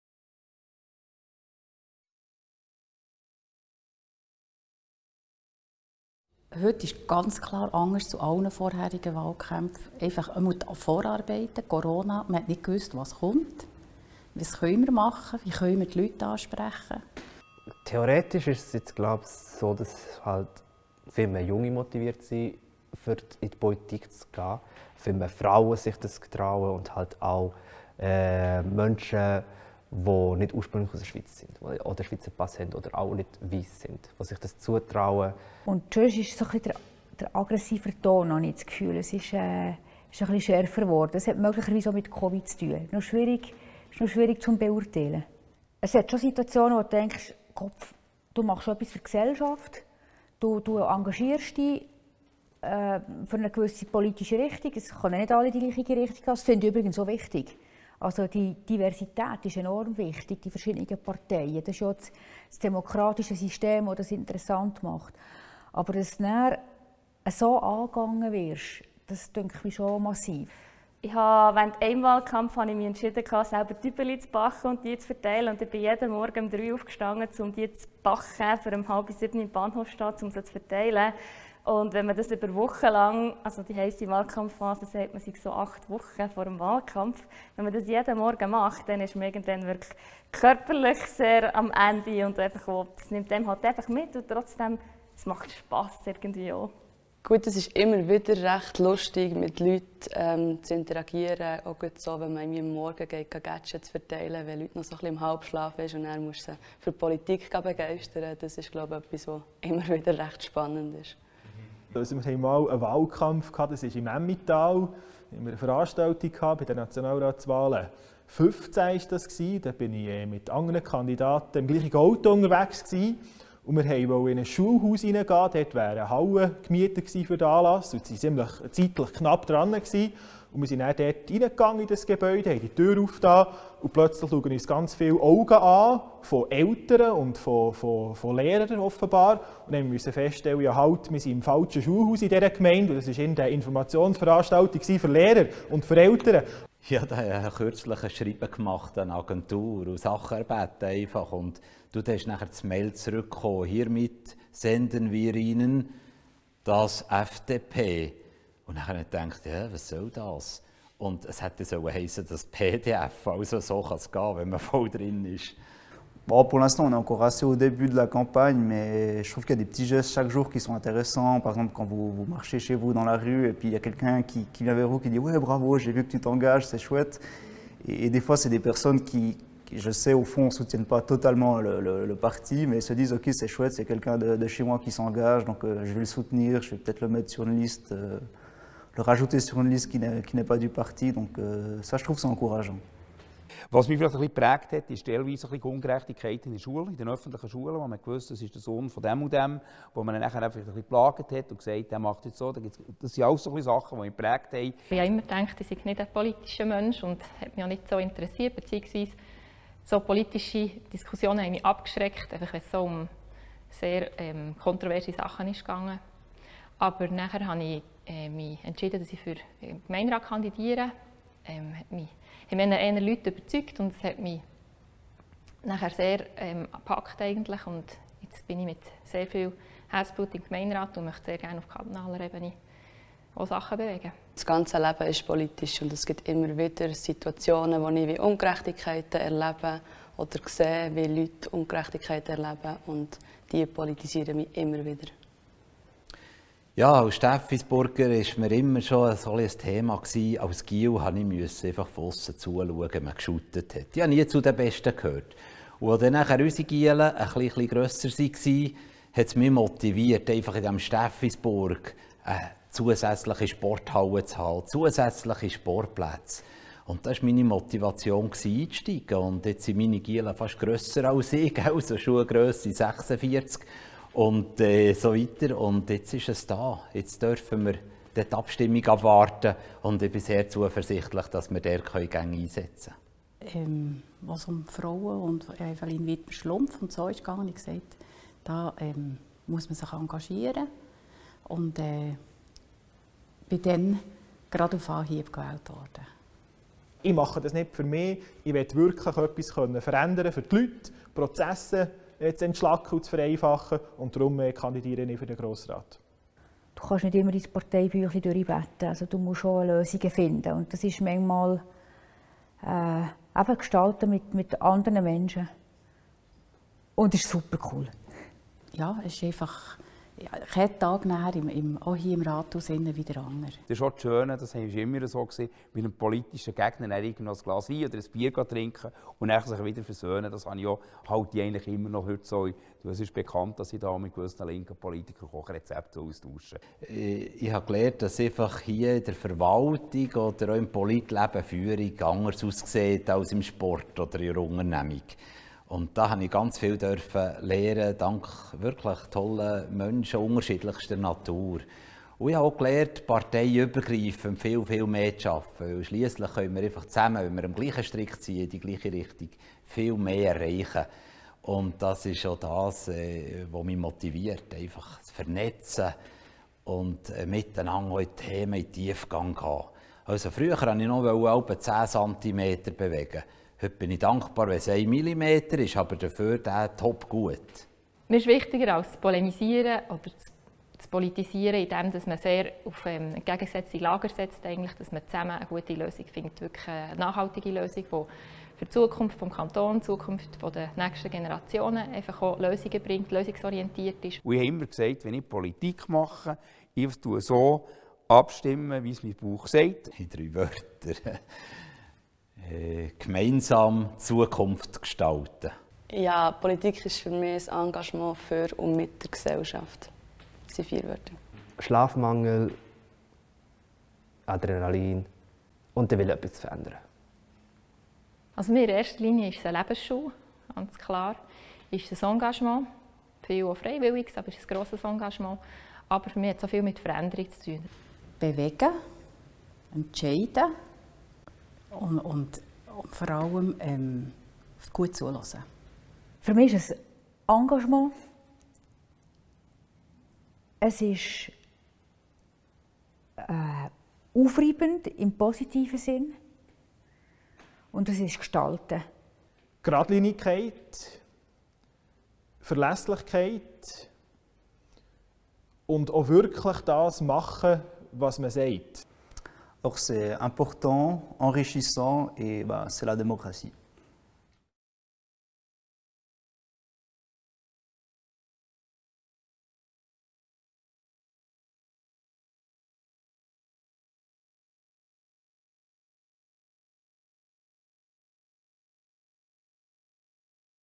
Pourquoi m'engager en faveur de la démocratie?
Chancellerie d'État du canton de Berne

10 candidates et candidats reviennent sur leur motivation et la campagne électorale 2022.